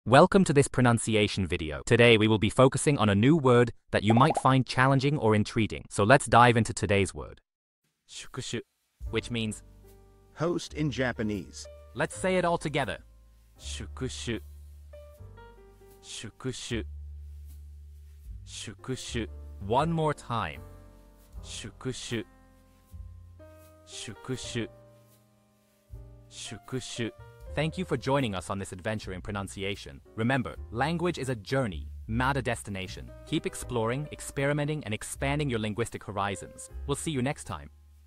How to pronounce yadonushi | 宿主 (Host in Japanese)
Master the Pronunciation of 'yadonushi | 宿主' - which means : Host in Japanese 📝 with @PronunciationJapanese 🎓🗣️ - [Your Guide to Japanese]

Learn to pronounce 'yadonushi' perfectly in Japanese with our easy and engaging tutorial by @PronunciationJapanese! 📖📽️ In this comprehensive video, we provide you with native speaker examples 🗣️👂 and phonetic breakdowns 🎵 to ensure you get the pronunciation just right.